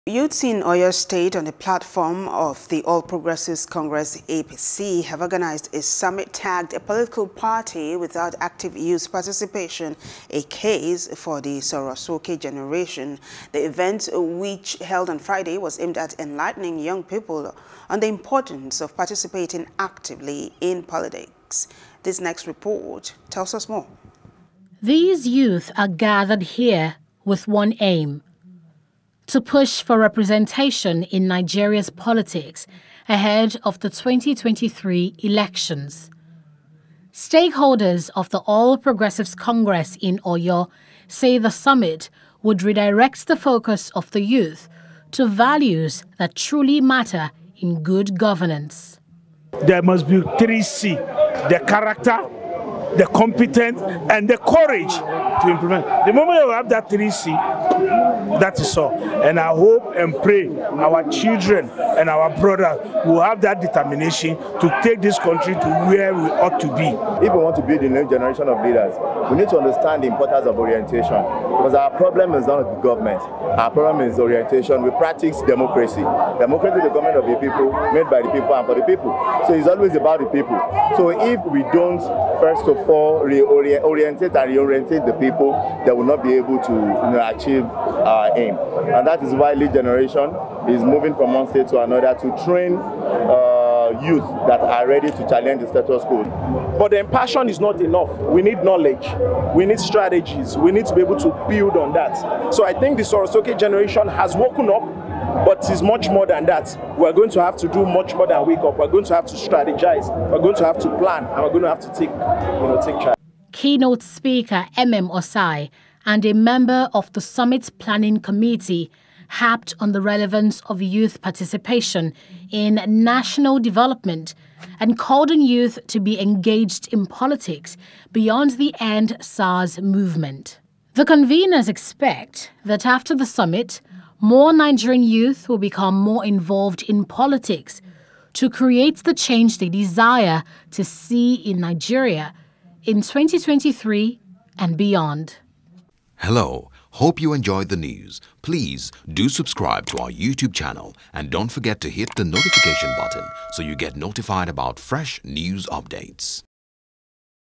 Stakeholders Task Youth On Political Participation Ahead of 2023 Elections | NIGERIA
Youths in Oyo state on the platform of the All Progressives Congress, APC, have organized a summit tagged “A Political Party Without Active Youth Participation; A Case for the Soro Soke Generation."
The event which held on Friday, was aimed at enlightening young people of the importance of participating actively in politics.